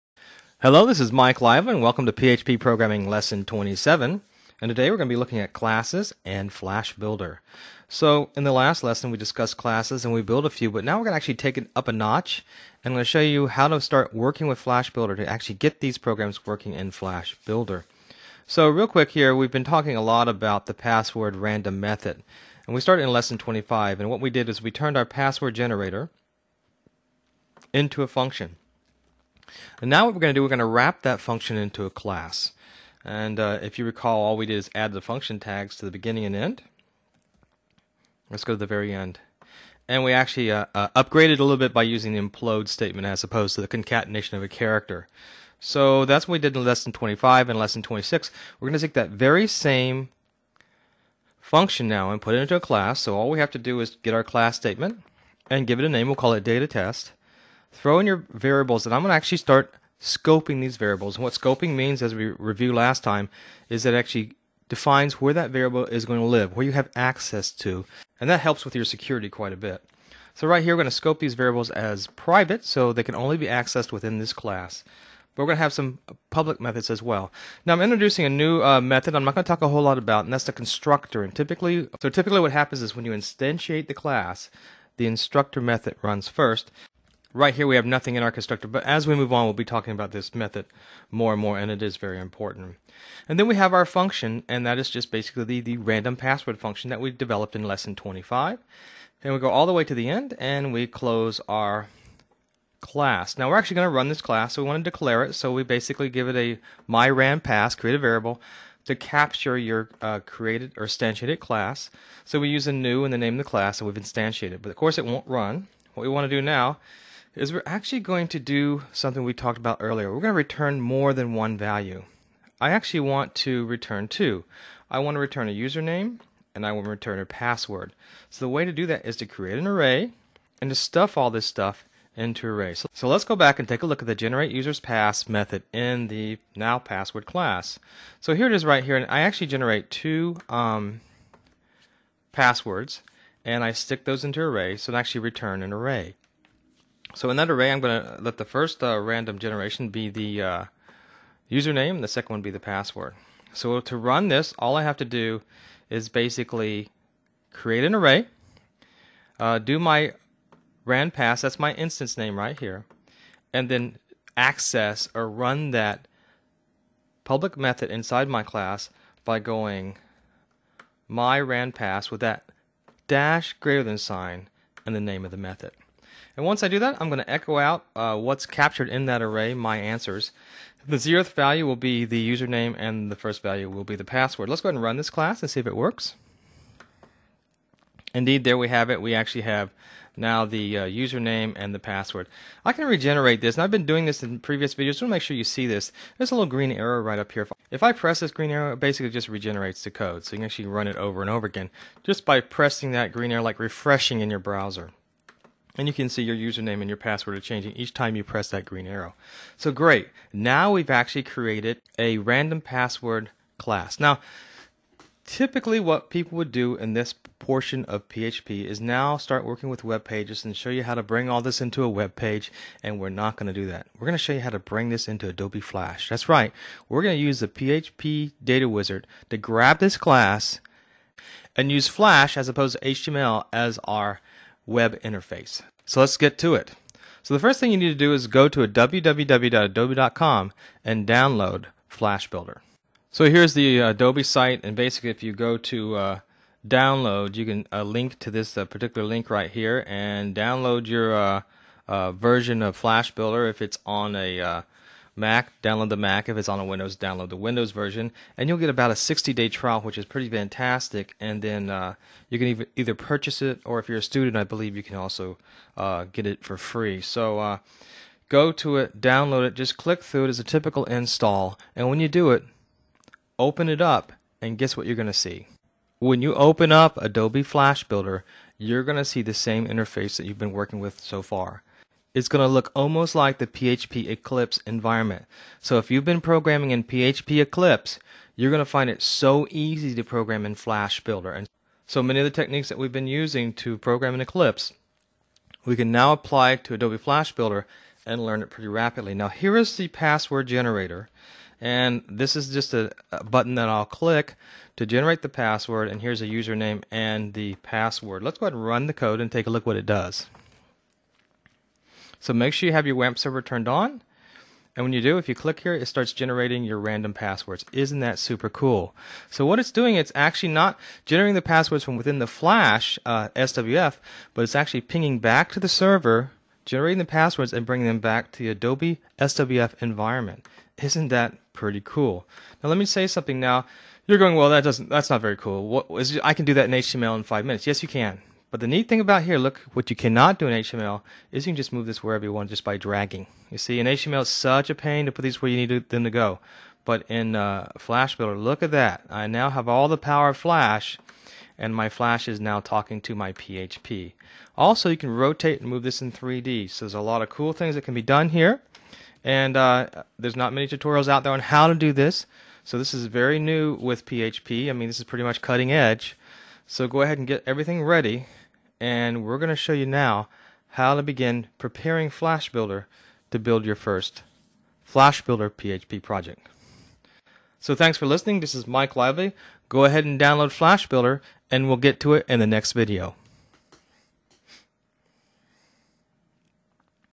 PHP Programming 27 Classes 2 and Flash Builder
In this tutorial you create a random password generator class for Flash Builder. This tutorial was created by Mike Lively.